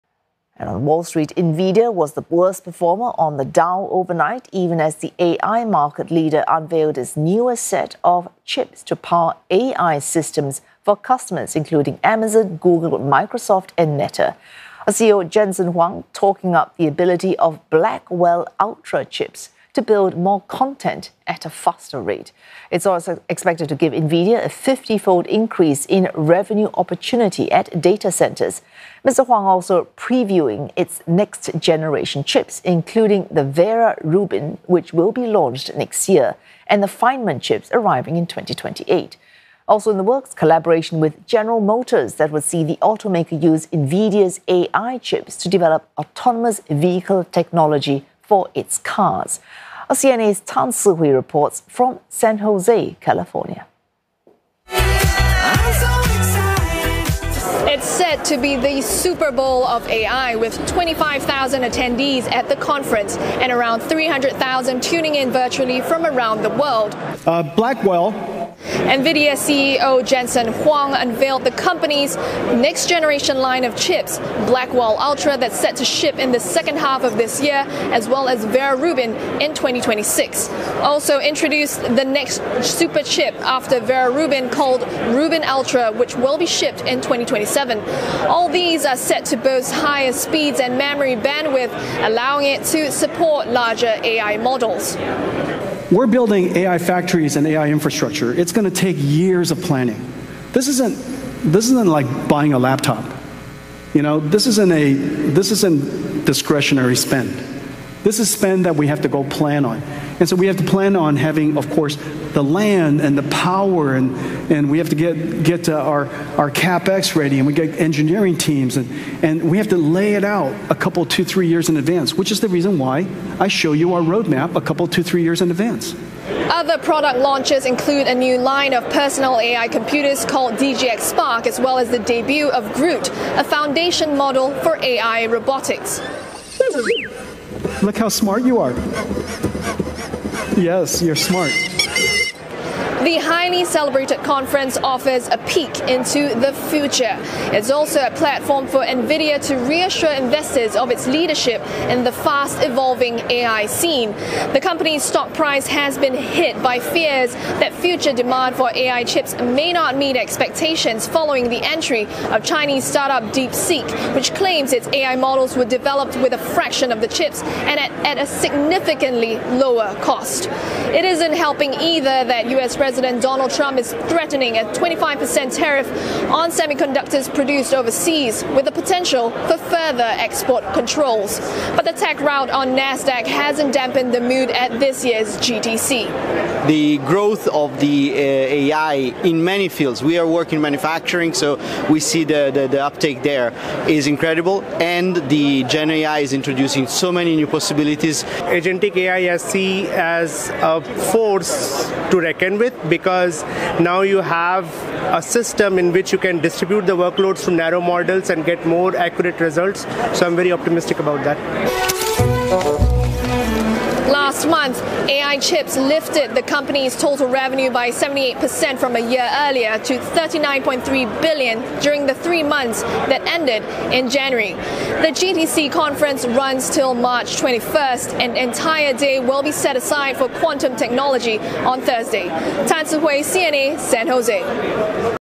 Nvidia unveils new AI superchips Blackwell Ultra, Vera Rubin
On Wall Street, Nvidia was the worst performer on the Dow overnight, even as the artificial intelligence (AI) market leader unveiled its newest set of chips to power AI systems for customers like Amazon, Google, Microsoft and Meta. CEO Jensen Huang talked up the ability of Blackwell Ultra chips to build more content at a faster rate. Mr Huang also previewed its next-generation chips, including the Vera Rubin which will be launched next year, and the Feynman chips arriving in 2028. CNA's Tan Si Hui reports from San Jose, California.